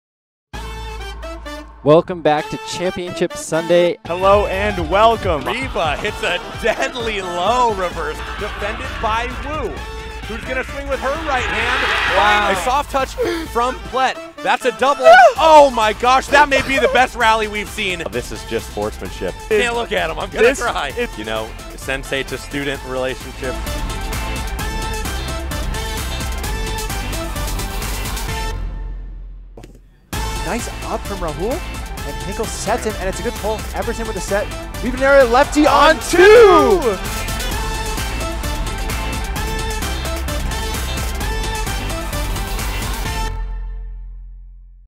2022 Spikeball Tour Series Stream Teaser Trailer
Grab your popcorn and get ready for more LIVE roundnet coverage of the 2022 Spikeball Tour Series!